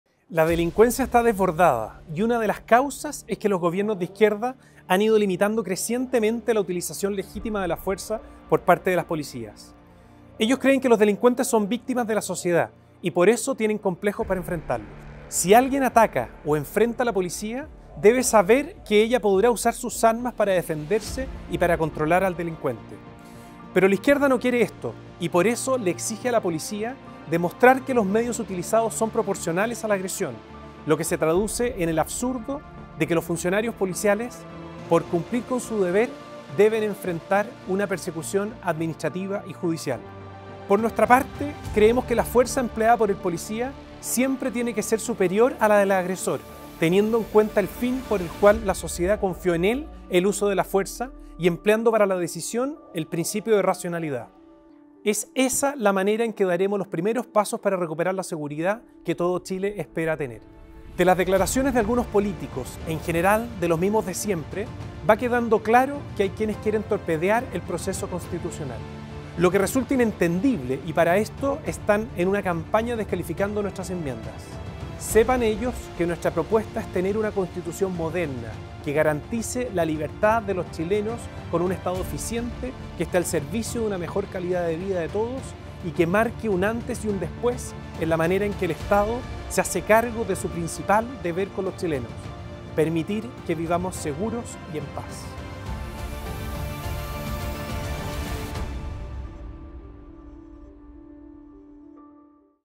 Nuestro presidente A.Squella se refiere a la actual situación de seguridad en nuestro país.
Los Republicanos creemos que la fuerza de las policías debe ser superior a la del agresor y es por esto, que no permitiremos que la izquierda le quite el derecho a nuestras Fuerzas Armadas y de Orden a proteger a los chilenos.